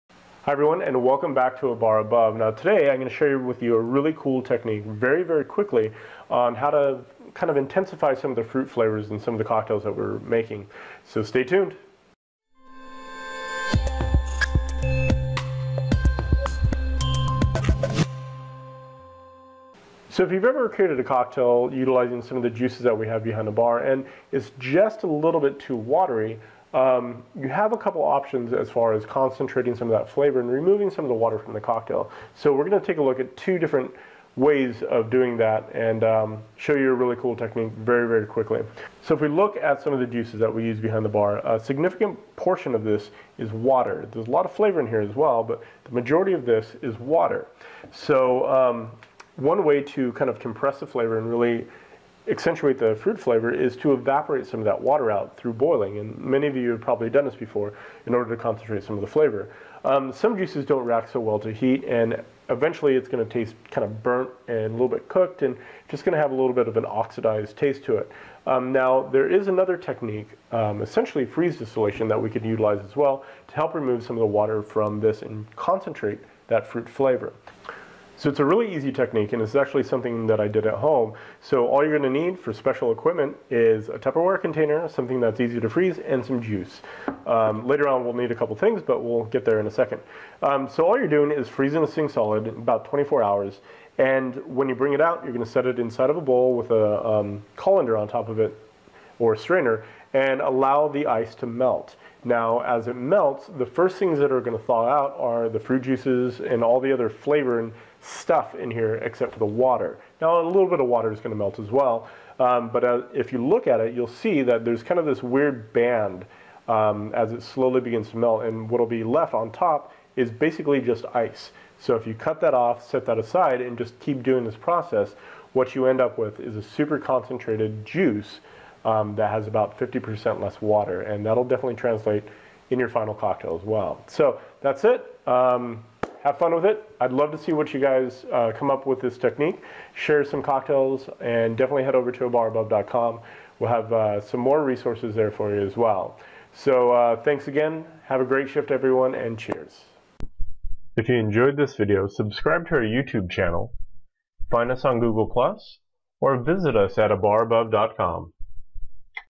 Probably the “Coolest” Technique to Date- Creating Fruit Concentrates
Here is an easy technique to add more flavor to your cocktails. DIY-fruit juice concentrates.